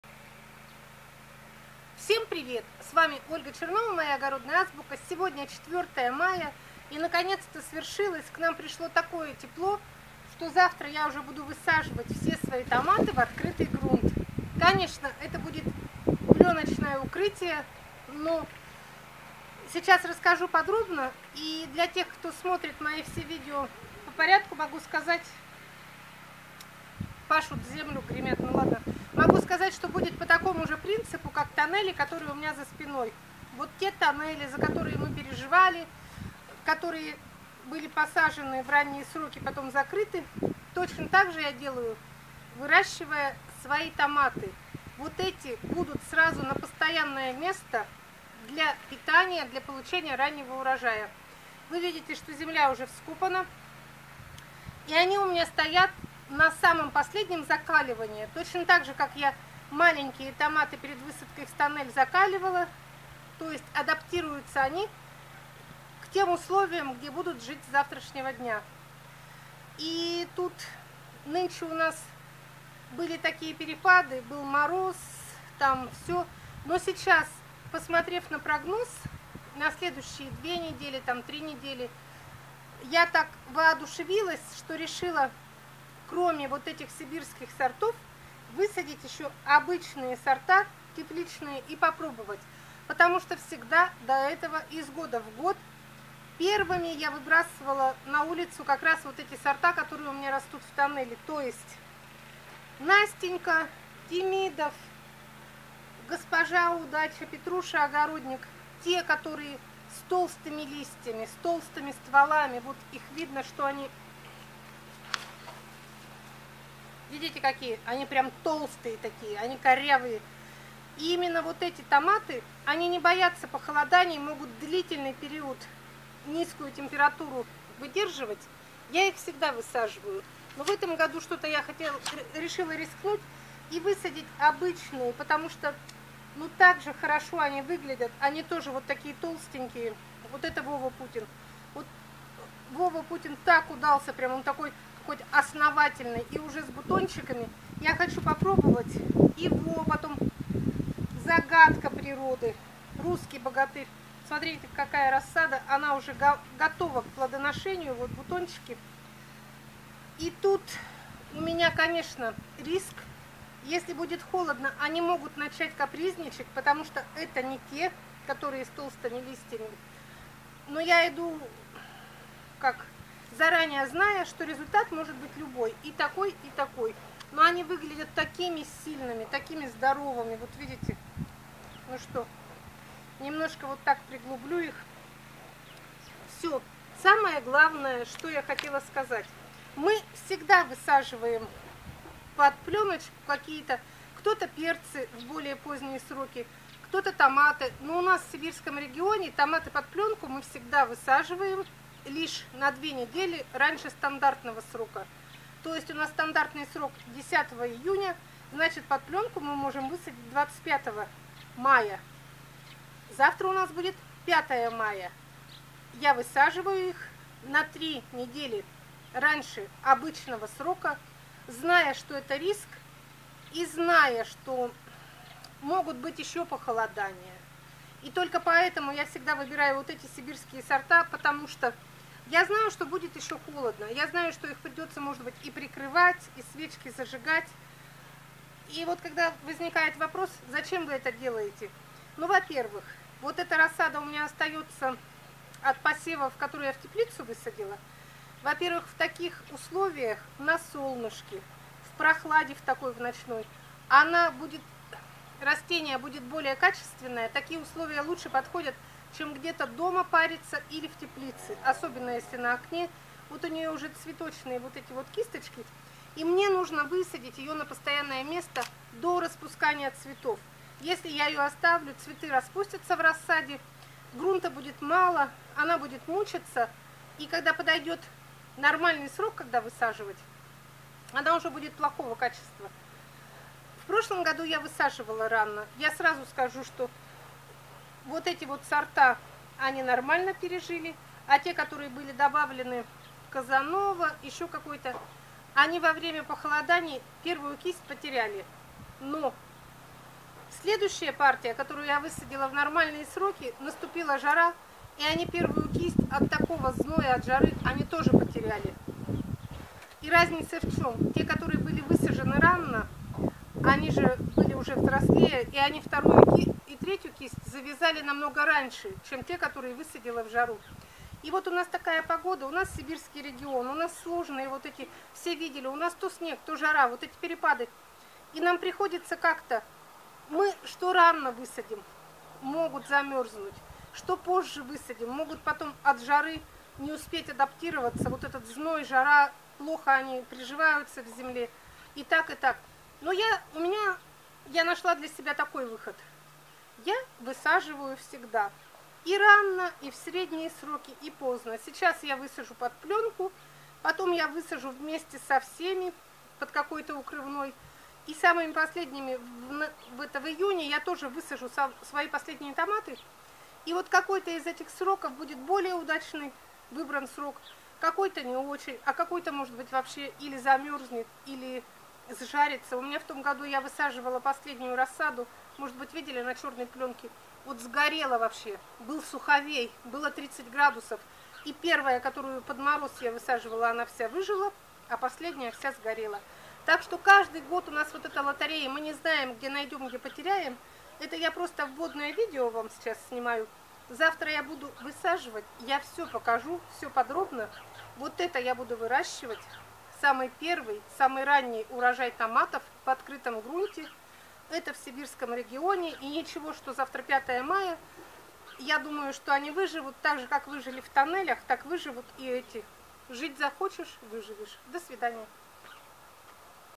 5 МАЯ - ТОМАТЫ В ОТКРЫТЫЙ ГРУНТ! Ольга Чернова.
Когда высаживать томаты в открытый грунт? Все зависит от региона, от вашей погоды, от условий, которые вы можете создать для своих томатов в случае похолодания. Лично я рискну свои первые томаты высадить уже завтра.